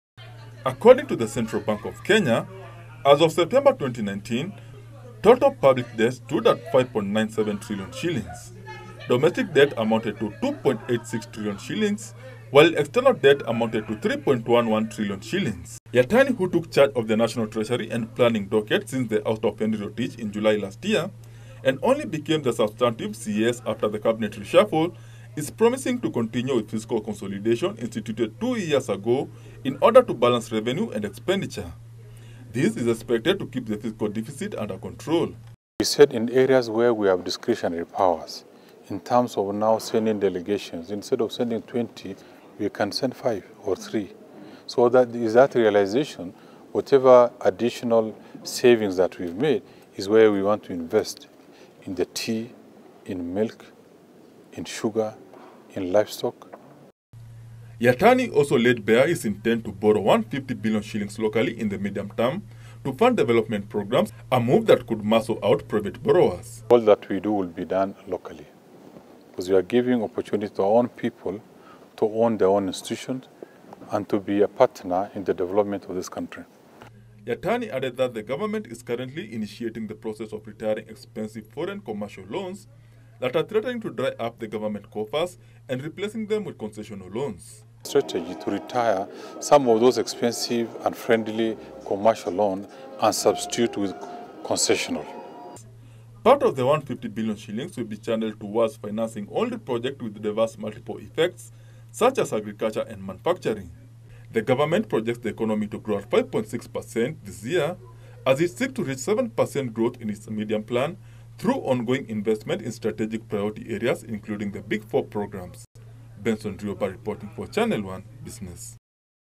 National Treasury
The National Treasury Cabinet Secretary  Ukur Yatani  has revealed plans to source 150 billion shillings from local lenders to finance development projects in the country in the medium term. The CS however says his focus going forward is to start restructuring public debt portfolio and retire expensive commercial loans replacing them with concessional loans in a bid to free up funds for development. Yatani also expected the economy to grow 5.6 percent this year.